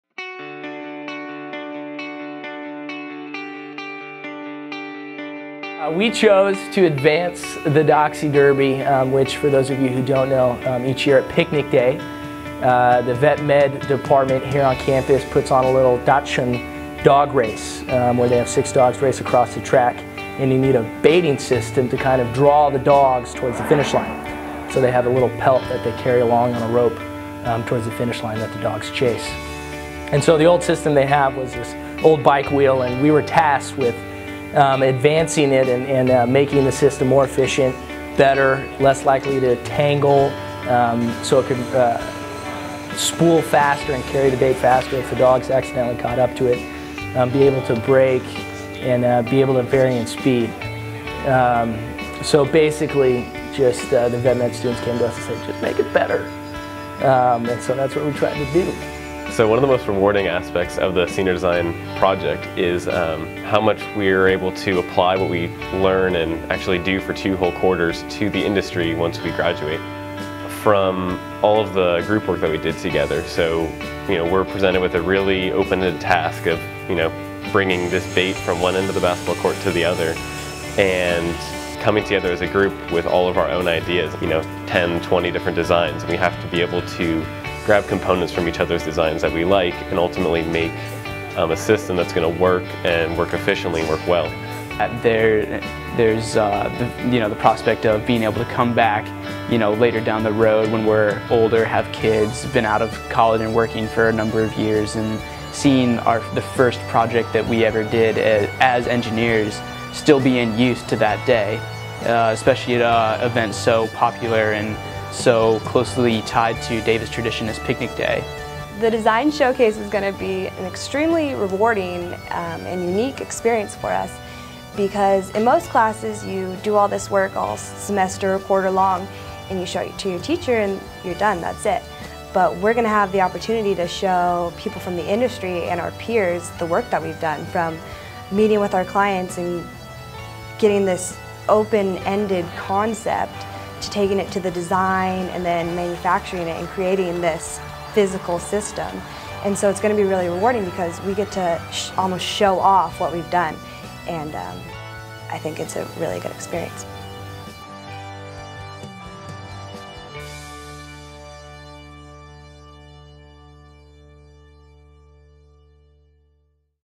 Engineering Design Showcase: The Doxie Derby Team
Every year, UC Davis undergraduate engineering students have the opportunity to display their work at an engineering design showcase in June. This year, a team of mechanical engineers, by the request of the UC Davis Veterinary Medicine school, improved the out-of-date pulley system used during the much-adored Doxie Derby event on Picnic Day.

Special thanks to Zac Dillow, Julie Jackson, Taylor Stein, and Robert Mitchell for their participation in this video.